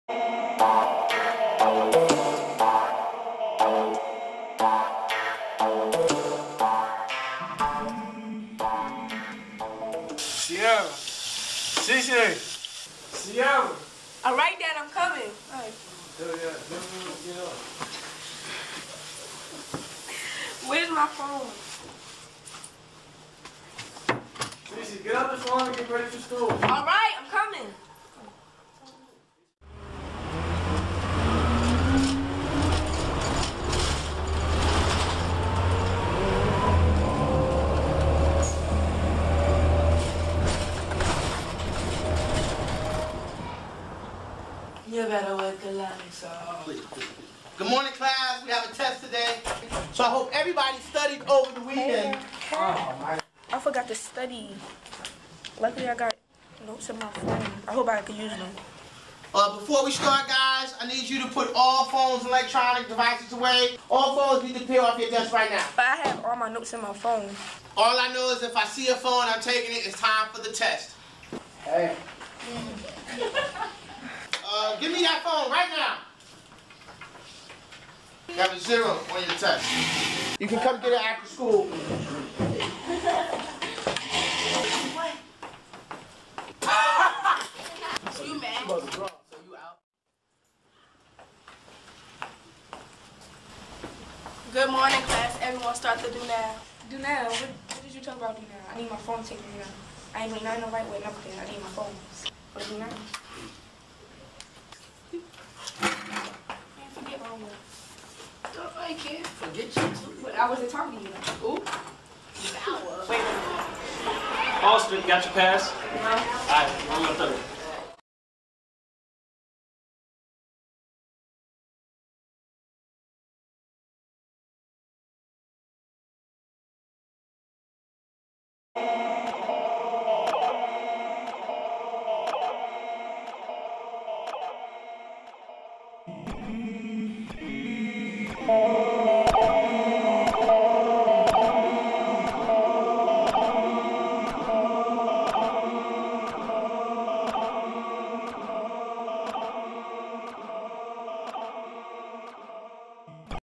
Phone Zombie
Central HS YP, Get Right Productions delivers a short about a young lady seemingly unable to cope or perform without her cell phone